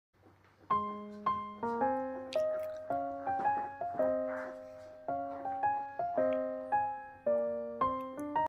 Newborn puppy had a face massage for the first time
My Cute little toby bear.

Hi friends. My name is TOBY the chocolate labrador. My humans call me MR. TOBY BEAR.

Your Lab,
MR. TOBY BEAR